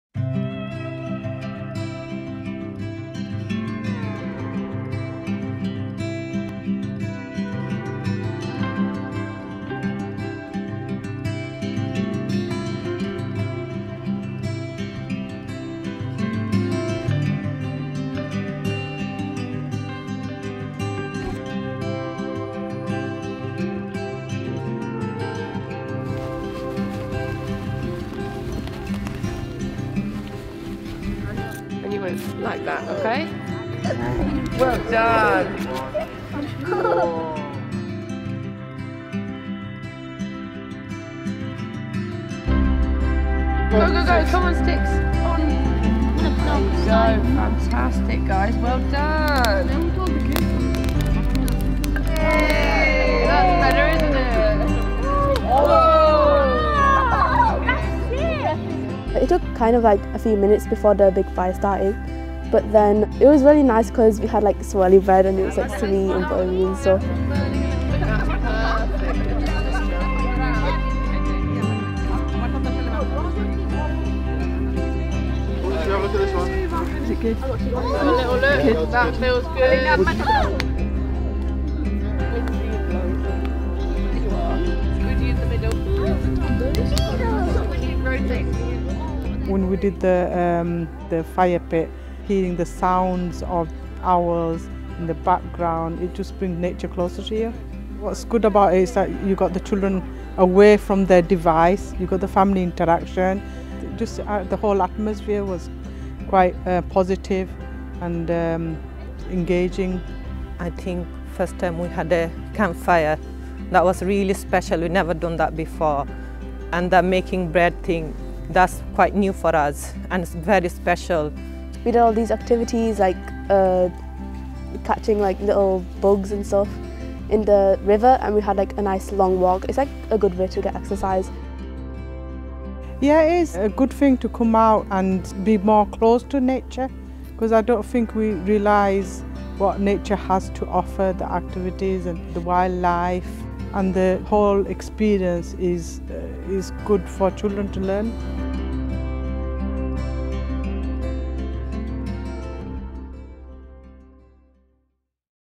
National Park Walks - Yorkshire Dales Dark Sky Reserve
There are many different ways to access and enjoy a walk out in the Yorkshire Dales National Park and you don't always have to come for a hike!

The Yorkshire Dales is home to some of the darkest skies in the country. We’re proud to have large areas of unpolluted night sky where it’s possible to see the Milky Way, planets, meteors and even the Northern Lights. And in 2020 The Yorkshire Dales National Park was designated a Dark Sky Reserve

These students joined us in the National Park during our annual Dark Skies Festival to enjoy the wonder of the night sky and to spend time in nature*.

Spending time in green space or bringing nature into your everyday life can benefit both your mental and physical wellbeing, so if this inspires you to come for a walk in the countryside you can find information and ideas on our website.

*This was filmed at an officially organised Yorkshire Dales National Park Authority event with Authority staff in attendance. We ask that people don't light fires (including BBQ's) in the National Park at any other time.